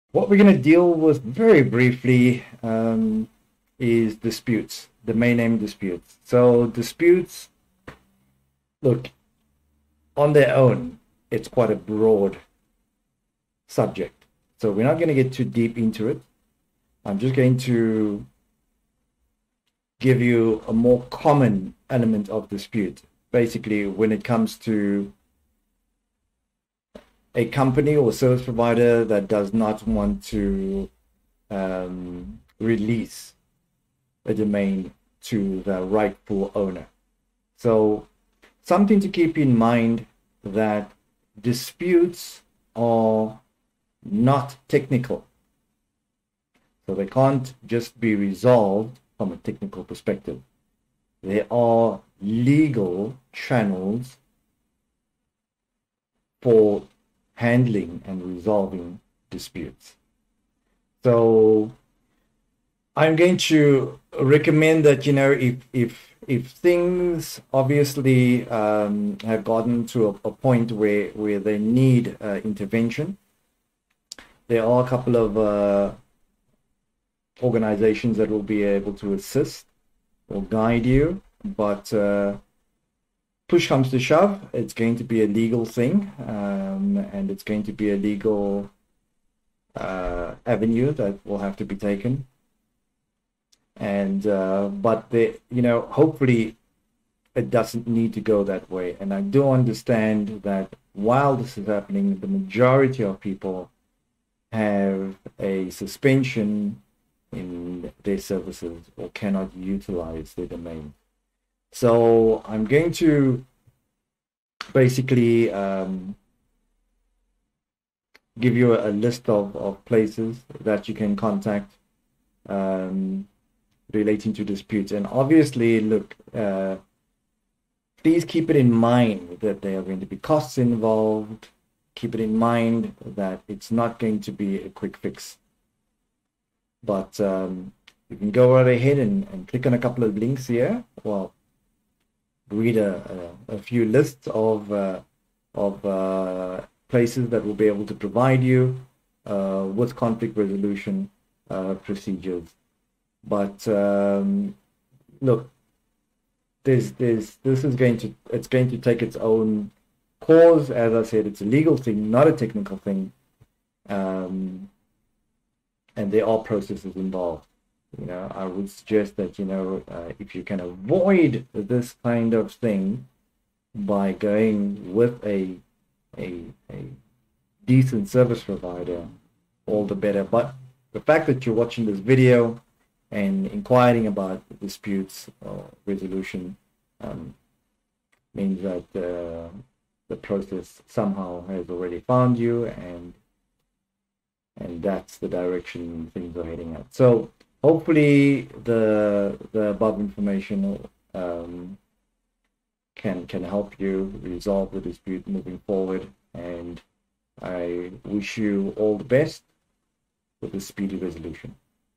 Domain Name Disputes
In this video, we go over domain name disputes and the various bodies you can work with to resolve them.